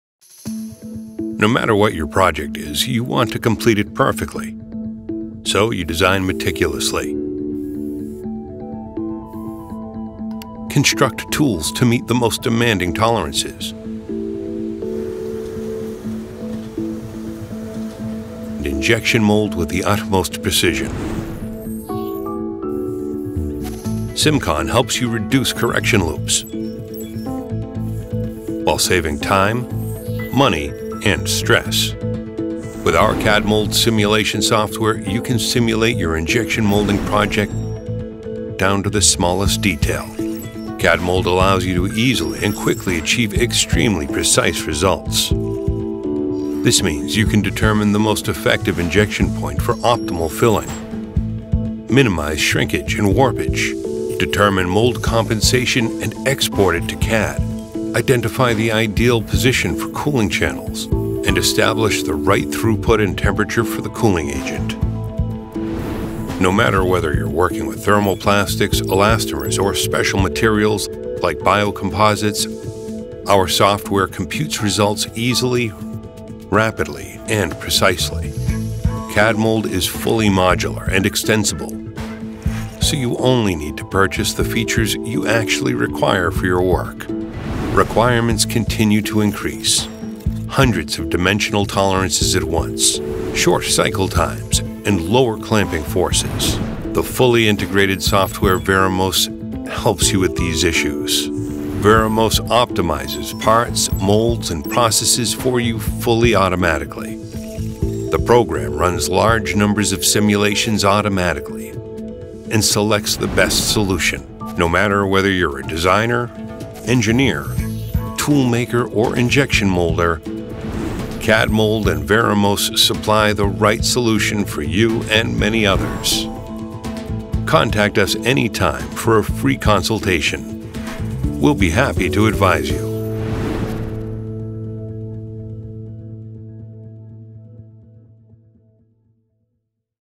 Better Injection Molding With Simulation Software Cadmould & Varimos by Simcon
Discover our injection molding simulation and optimization software and visit our

Precise predictions of part behavior reduce the number of correction loops required by 25 percent and cut your material costs at the same time.

The high-precision simulation results for CADMOULD facilitate decision-making in the development of complex plastic parts. This ensures that all the decisive criteria can be configured in an optimal manner.
It enables you to
- shorten cycle times
- predict and reduce shrinkage and warpage
- prevent shrink marks, air traps and weld lines

VARIMOS automates the design of experiments and thus ensures better plastics parts, molds and processes. The software independently and systematically simulates a large number of specified process and geometry variables for the injection molding process.